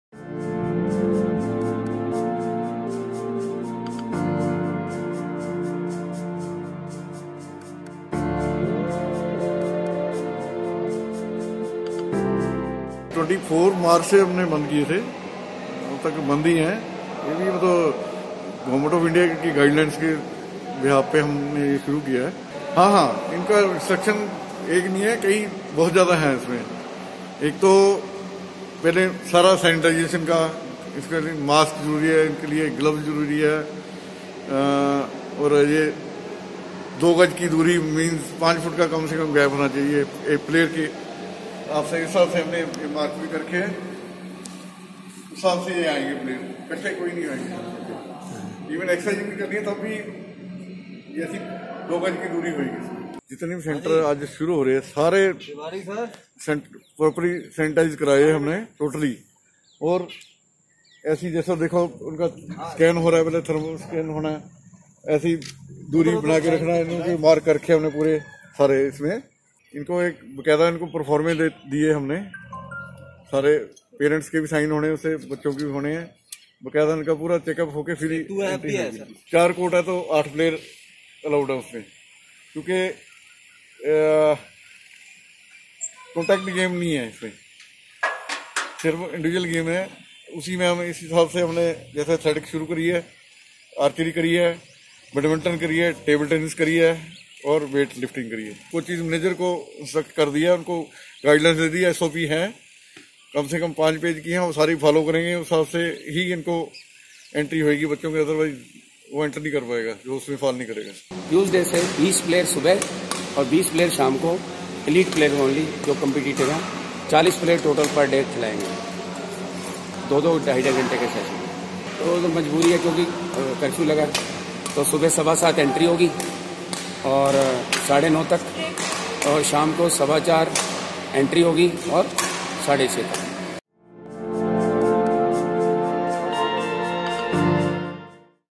After two months the Chandigarh Sports Department has opened its complexes for sportspersons
After nearly 60 days, the UT Sports Department opened its complexes for sportspersons today.

Partial facilities at the sports complexes of Sector 38 (West), Sector 7, Sector 34, Sector 42 and Lake Sports Complex have been allowed in the first phase as per the manual issued by the department. Trainees of various disciplines, including badminton (Sector 38 sports complex), athletics (Sector 7), table tennis (Sector 50, Sector 34 and Sector 23), archery, tennis and weightlifting (Lake Sports Complex and Sector 42) attended session in small groups today.